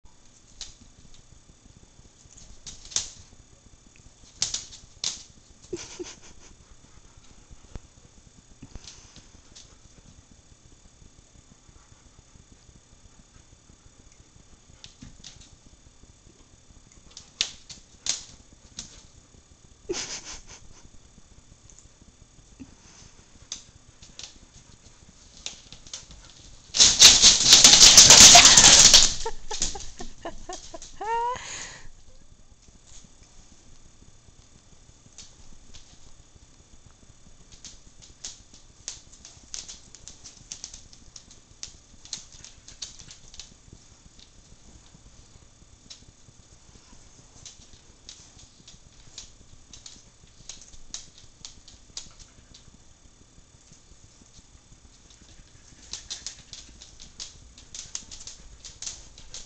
my siberian nut job,Gus
he is the best dog in the world, not the brightest, but damn cute